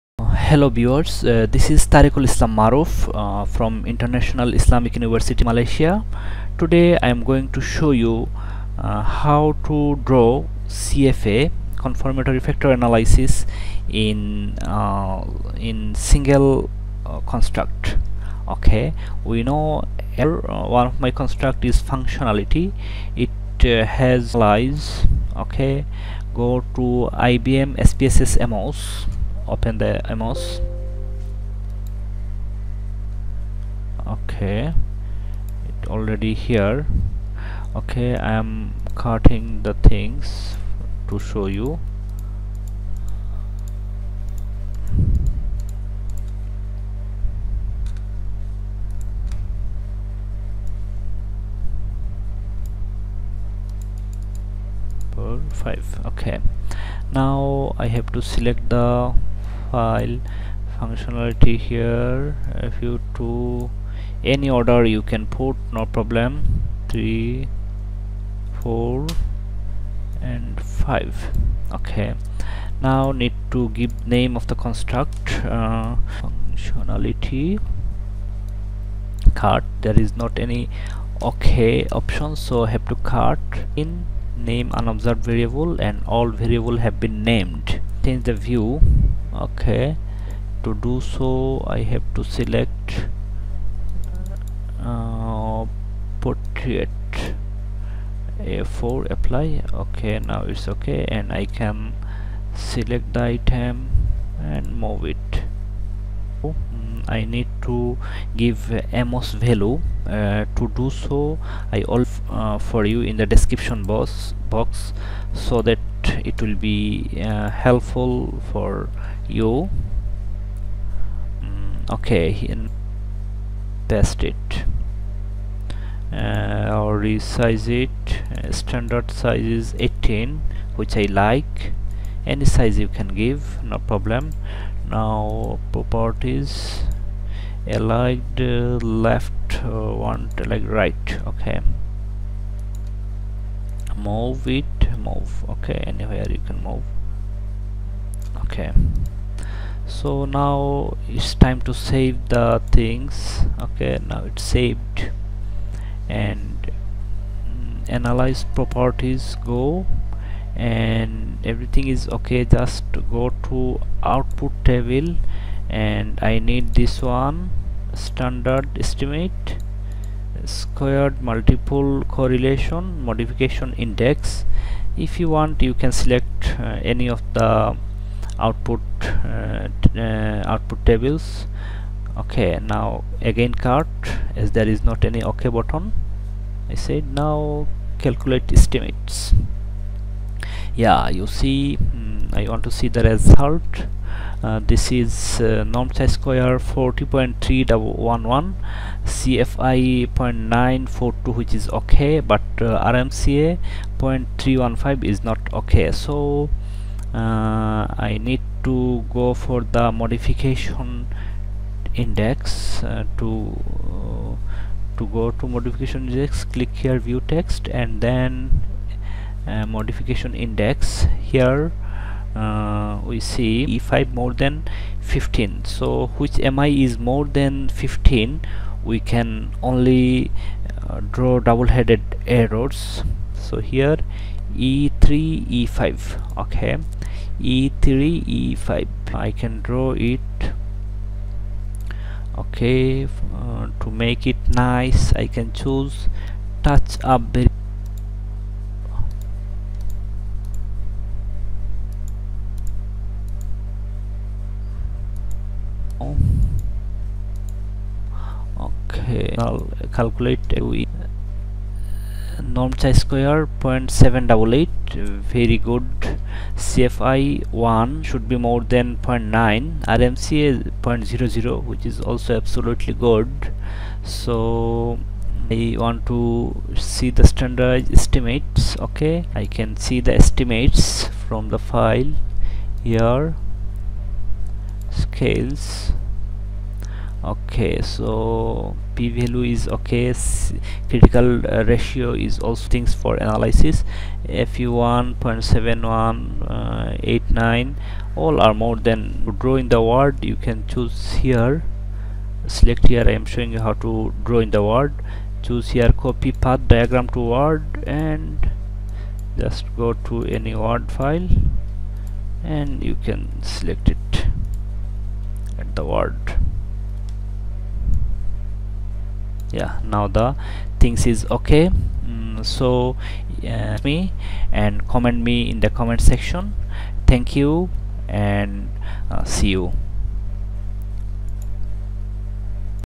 HOW TO DO CONFIRMATORY FACTOR ANALYSIS SPSS AMOS WITH SINGLE CONSTRUCT
This video provides a brief overview of how to use AMOS (structural equation modeling program) FOR BIGINERS to carry out confirmatory factor analysis of survey scale items of single construct.

Amos Value:
Chi Square=\cmin
df=\df
Normed Chi Square=\cmindf
CFI=\cfi
RMSEA=\rmsea